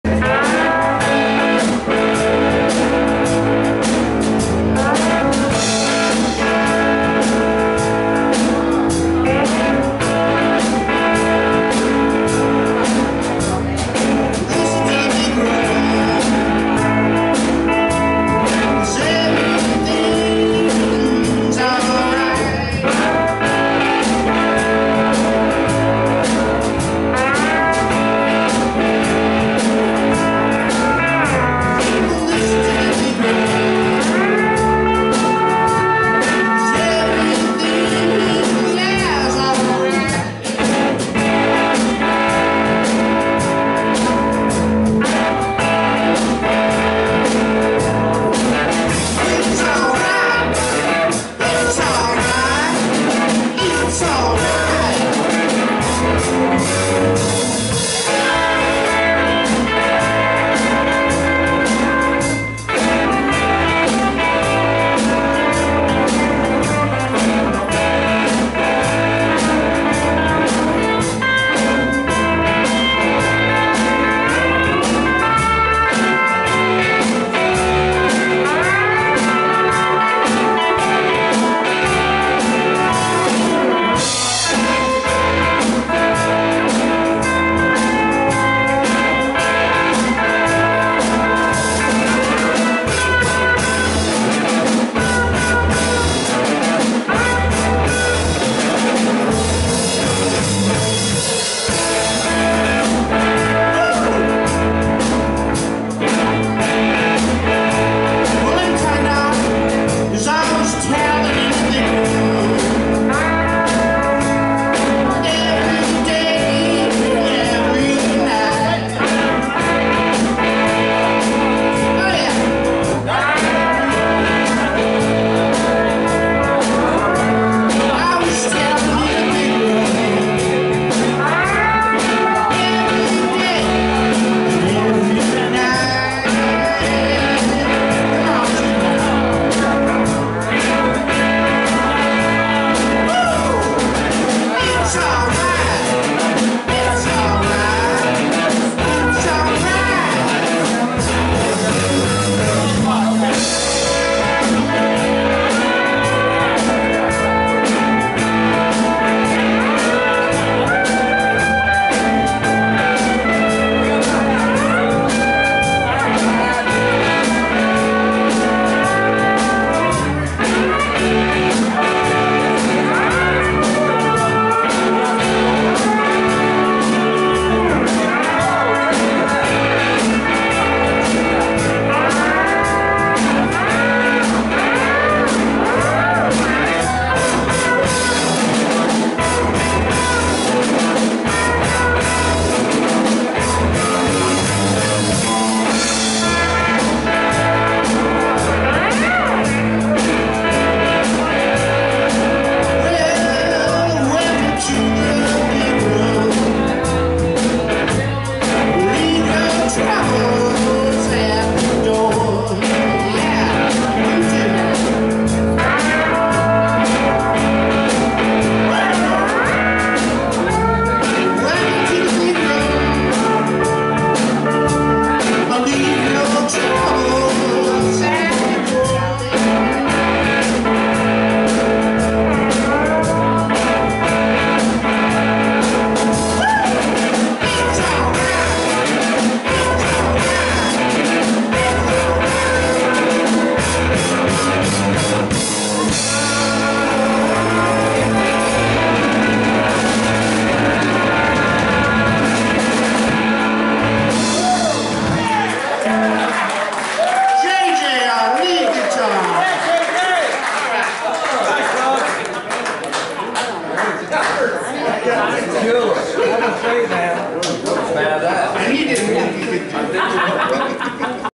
Welcome to the Big Room
a jam at Hambones in Clarksdale, Mississippi on May 23, 2019.